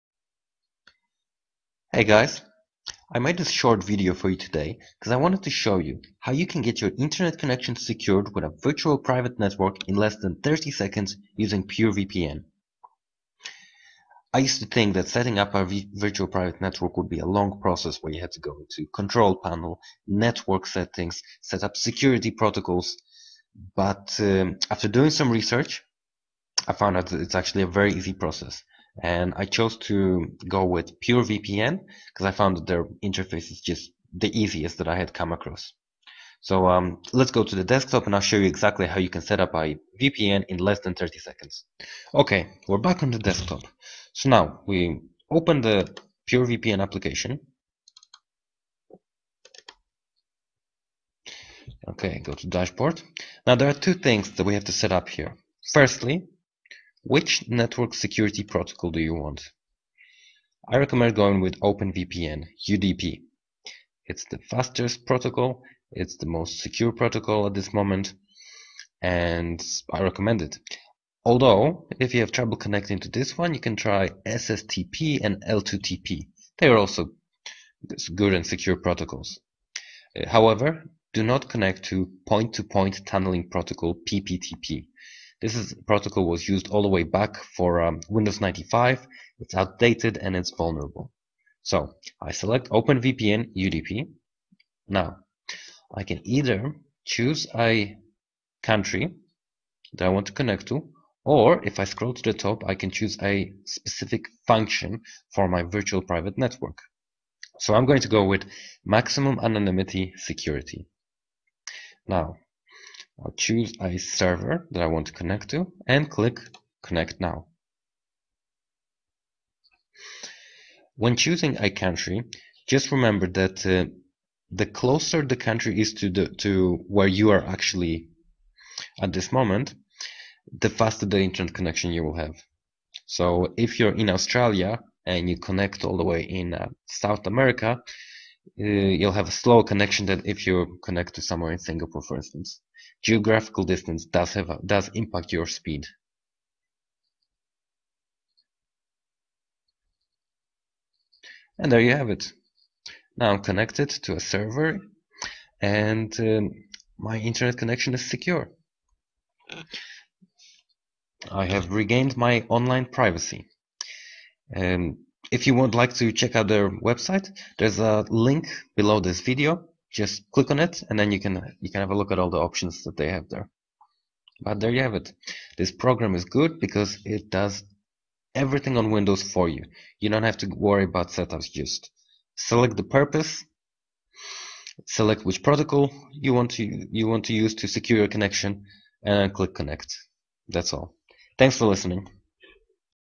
Setup VPN Windows 8 in less then 30 seconds using Pure VPN
I am using PureVPN software as it involves the easiest 3 step process for setting up a VPN connection. Here’s the link to the Pure VPN

“How to get your internet connection secured with a VPN in less then 30 seconds using Pure VPN” I have created a short video tutorial how you can setup VPN Windows 8.

I have found that it is the best software to hide your ip address online and regain your online anonymity and privacy.

In my example above I use the program to setup vpn windows 8, but it also works on windows 7, android and iOS. It’s compatible for all systems and it’s the best way to hide your ip address online.